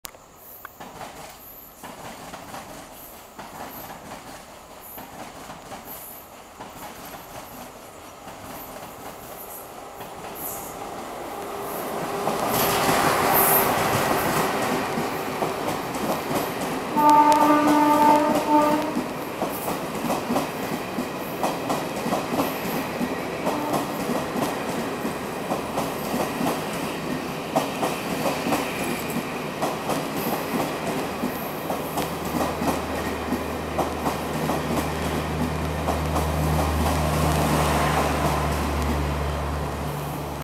[2009] ICF Rajdhani Mumbai-Ahmedabad AC Express
BL WCAM-2P hauled 2931 BCT-ADI ICF Rajdhani AC Express passes Andheri.